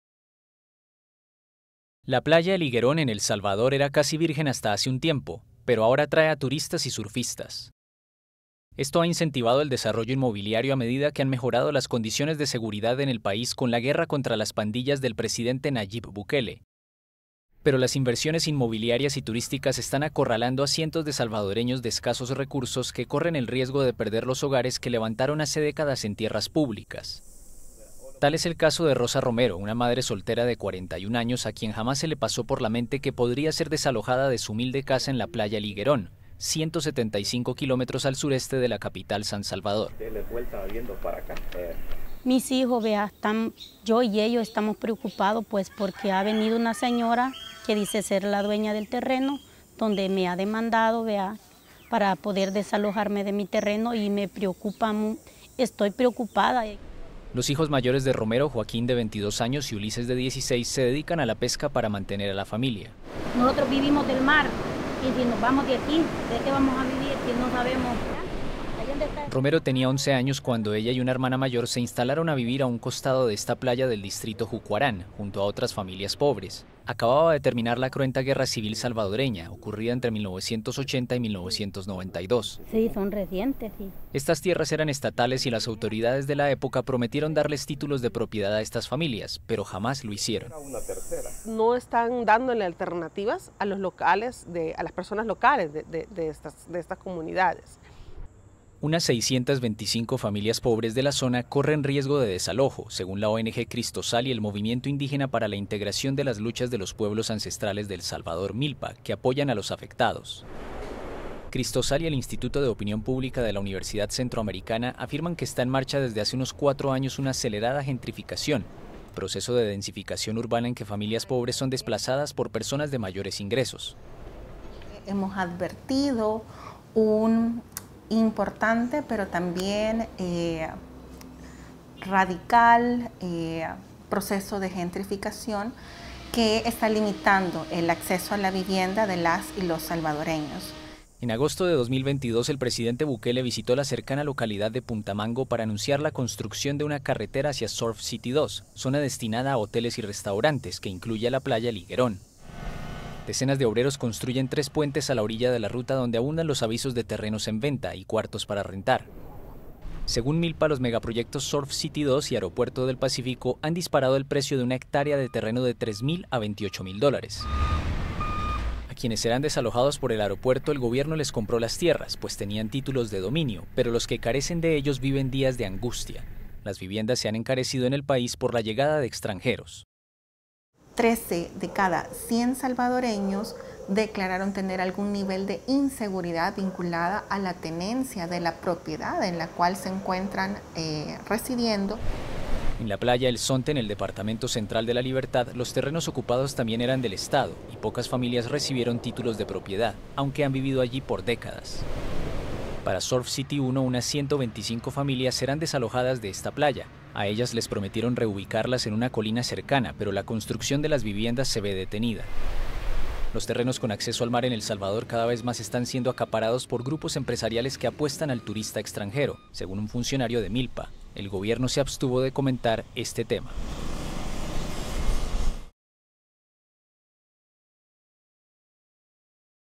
Megaproyectos inmobiliarios acosan a familias pobres en playas de El Salvador
A Rosa Romero jamás se le pasó por la mente que podría ser desalojada de su humilde casa en una playa de El Salvador. Pero las nuevas inversiones inmobiliarias y turísticas la han acorralado.